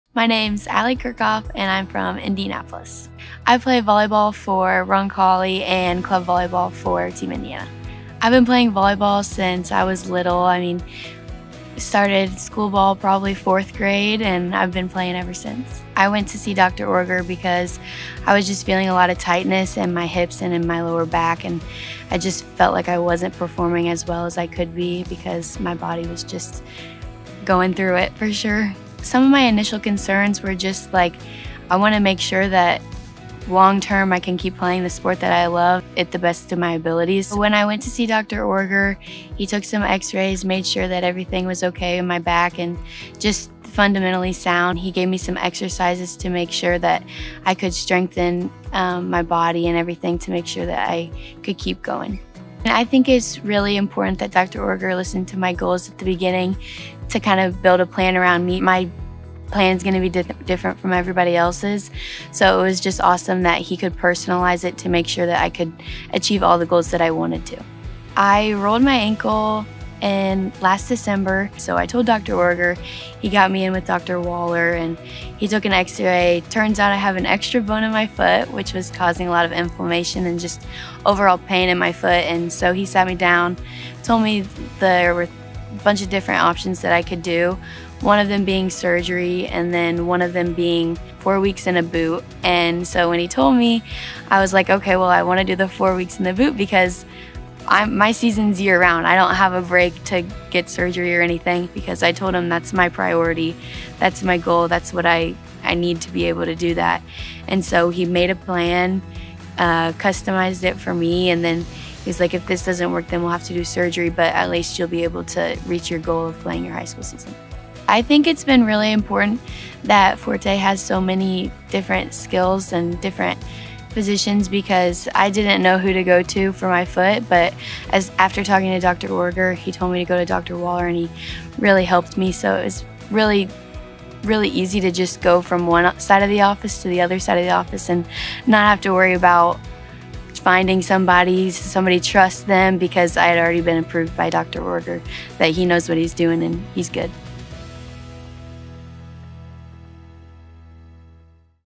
Forté Feature - Aly Kirkhoff
Playing volleyball for Roncalli High School and Team Indiana Volleyball means everything to Aly. When she started feeling pain in her back and ankle, her number one goal was to ensure she could still play the sport she loves.

Aly shares how Dr. Tony Origer, a chiropractic physician, and Dr. Andrew Wohler, a foot and ankle orthopedic surgeon, provided personalized treatment plans to help her accomplish that goal.

Learn how we help our patients like Aly get back to what they love doing most